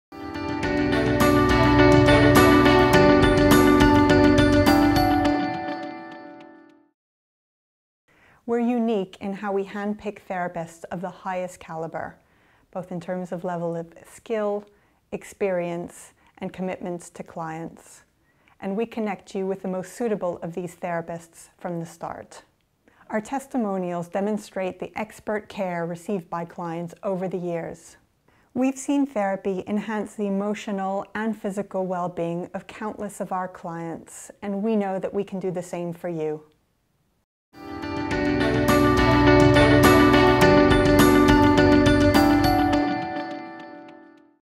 Why choose Harley Therapy? What sets Harley Therapy apart from others?
There are many therapists to choose from so why choose Harley Therapy? What sets us apart?

In this video, Dr Sheri Jacobson, Clinical Director of Harley Therapy, describes how therapists are hand picked based on their experience, skill and committment.

You will have a therapist suggested to you that is the most suitable for your needs.

Over the years, Harley Therapy has received some wonderful testimonials. "We've seen therapy enhance the emotional and physical well-being of countless of our clients and we know we can do the same for you".